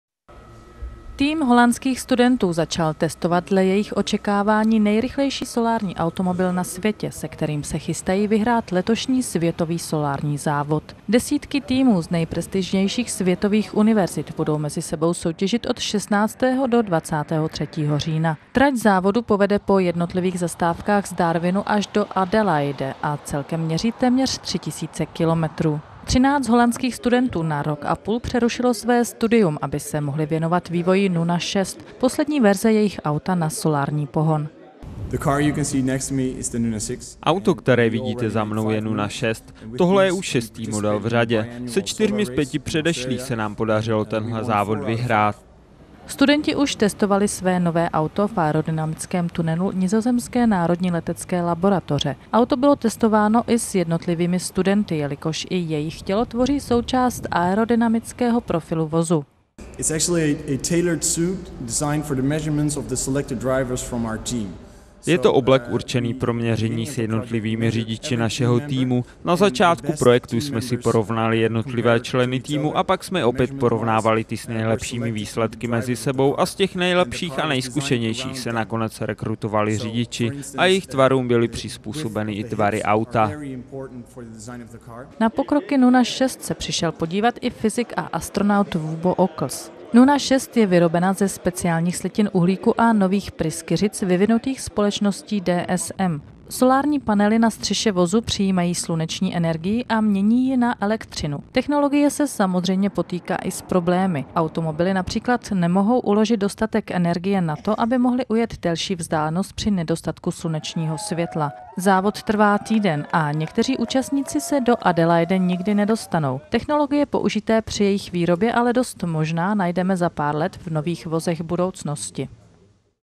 Zprávy NTD - Holandští studenti se připravují na 5. ročník Světového solárního závodu
Tým holandských studentů začal testovat dle jejich očekávání nejrychlejší solární automobil na světě, se kterým se chystají vyhrát letošní Světový solární závod.

Desítky týmů z nejprestižnějších světových univerzit budou mezi sebou soutěžit od 16. do 23. října. Trať závodu povede po jednotlivých zastávkách z Darwinu až do Adelaide a celkem měří téměř 3000 kilometrů.

13 holandských studentů na rok a půl přerušilo své studium, aby se mohli věnovat vývoji Nuna 6 - poslední verze jejich auta na solární pohon.

Pier van Zonneveld, vedoucí týmu:
„Auto, které vidíte za mnou je Nuna 6. Tohle je už šestý model v řadě. Se čtyřmi z pěti předešlých se nám podařilo tenhle závod vyhrát."

Studenti už testovali své nové auto v aerodynamickém tunelu Nizozemské národní letecké laboratoře. Auto bylo testováno i s jednotlivými studenty, jelikož i jejich tělo tvoří součást aerodynamického profilu vozu.

„Je to oblek určený pro měření s jednotlivými řidiči našeho týmu. Na začátku projektu jsme si porovnali jednotlivé členy týmu a pak jsme opět porovnávali ty s nejlepšími výsledky mezi sebou a z těch nejlepších a nejzkušenějších se nakonec rekrutovali řidiči. A jejich tvarům byly přizpůsobeny i tvary auta."

Na pokroky Nuna 6 se přišel podívat i fyzik a astronaut Wubbo Ockels.

Nuna 6 je vyrobena ze speciálních slitin uhlíku a nových pryskyřic vyvinutých společností DSM. Solární panely na střeše vozu přijímají sluneční energii a mění ji na elektřinu. Technologie se samozřejmě potýká i s problémy. Automobily například nemohou uložit dostatek energie na to, aby mohly ujet delší vzdálenost při nedostatku slunečního světla. Závod trvá týden a někteří účastníci se do Adelaide nikdy nedostanou.

Technologie použité při jejich výrobě ale dost možná najdeme za pár let v nových vozech budoucnosti.